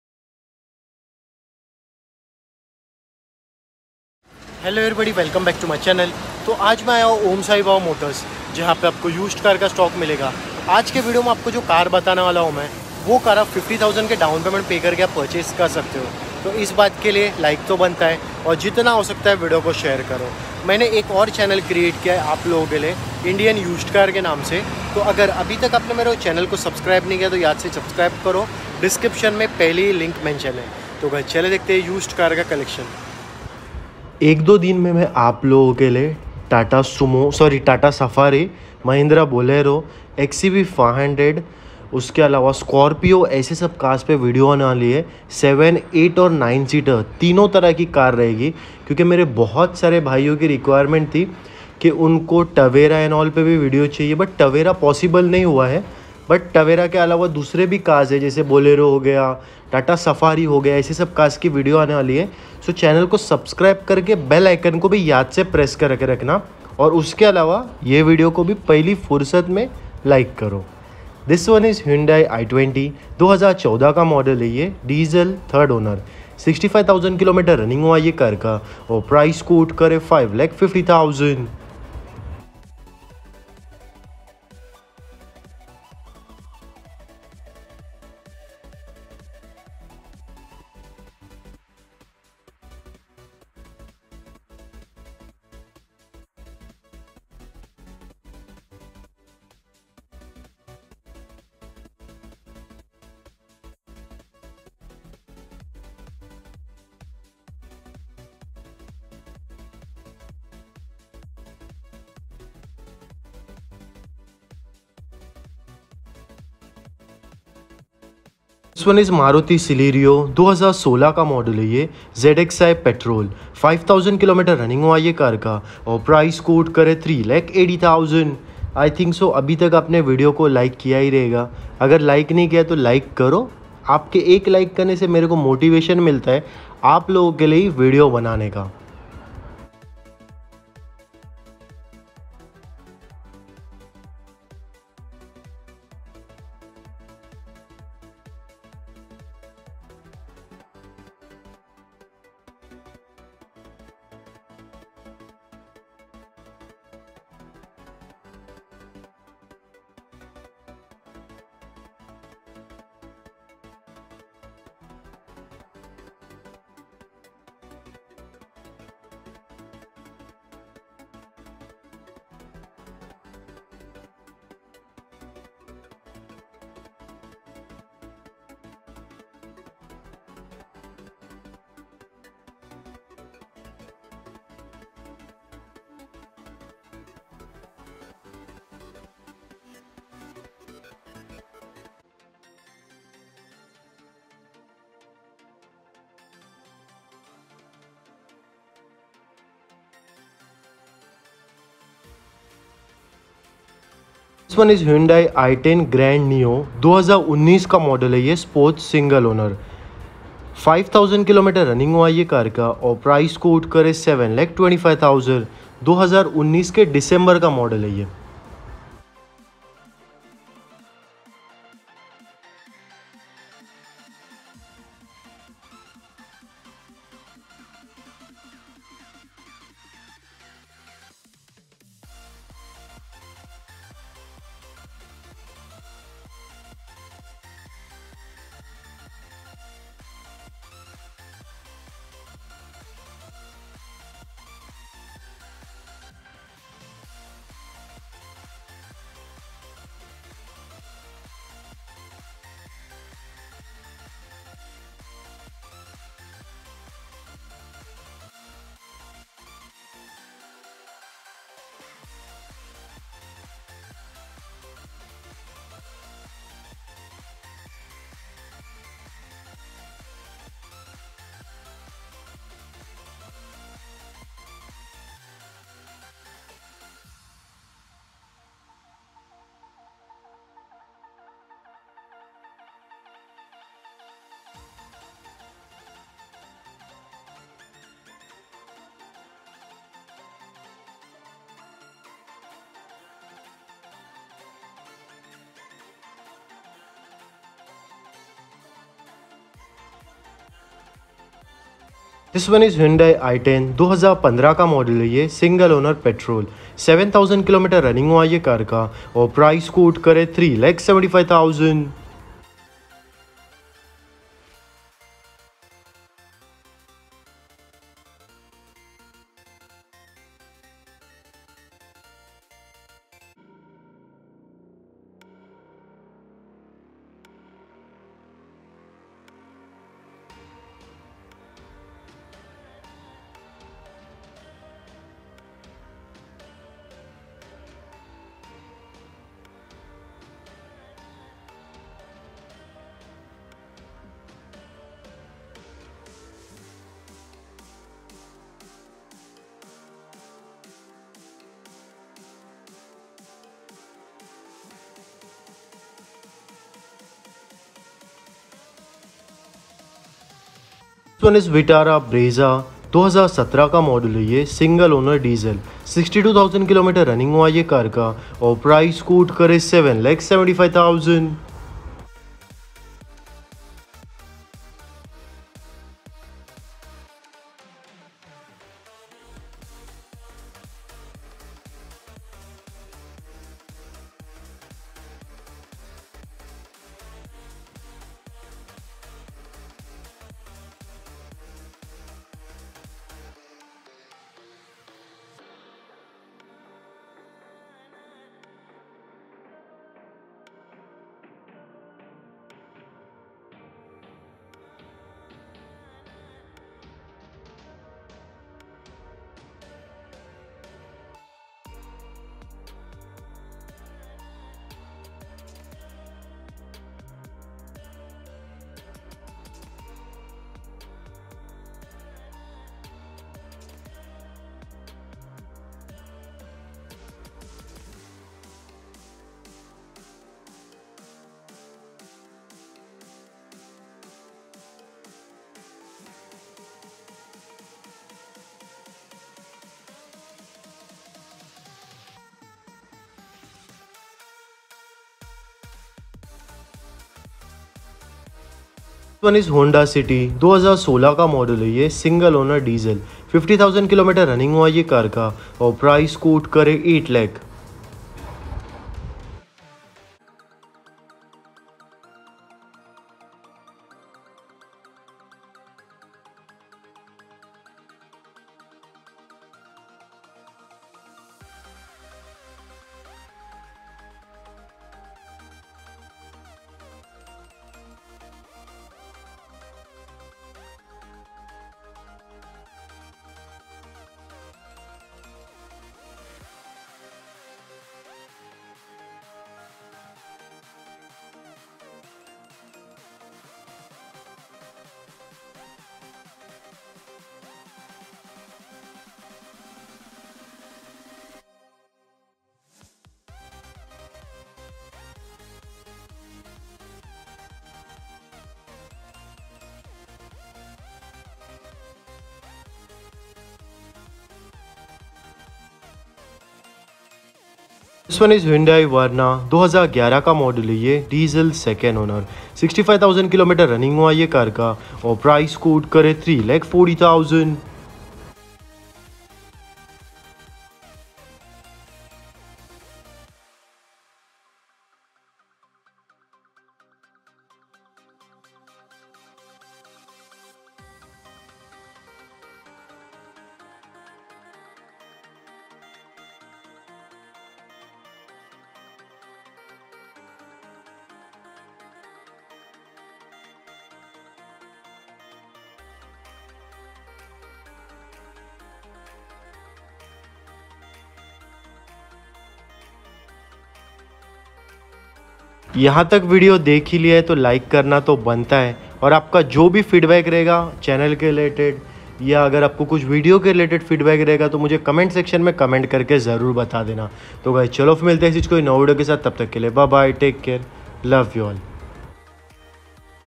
₹50,000 द्या आणि वापरलेली कार खरेदी करा Honda City, i10, Verna, i20, Brezza, Celerio | Fahad Munshi
Used Car in Ulhasnagar  | Maharashtra
Om Sai Baba Motors Ulhasnagar
Address- Kalyan-Ambernath Road, Shanti Nagar, Ulhasnagar-421003.Dist. Thane.

Contact Between - 11am to 6pm